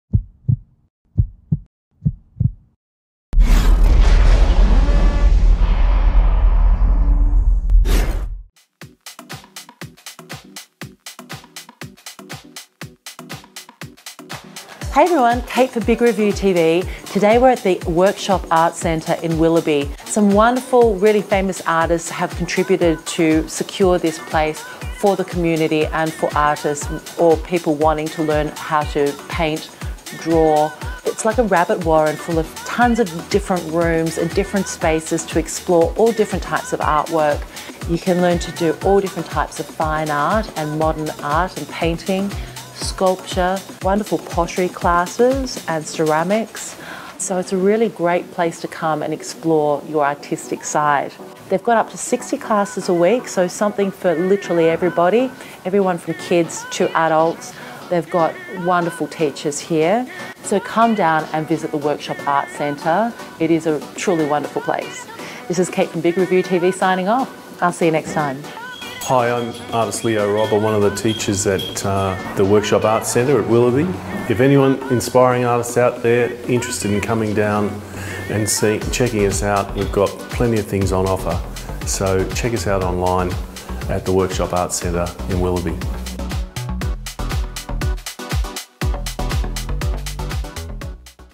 Workshop Art Centre in Sydney offering Painting Classes and Workshop
The Workshop Arts Centre (WAC) is an independent not-for-profit art centre and workshop facility. WAC users have access to a wide range of professionally equipped studios. There are three large adaptable studios used for writing, painting and drawing, as well as ceramics, printmaking, jewellery and sculpture studios.

Workshop Arts Centre
33 Laurel Street Willoughby NSW Australia 2068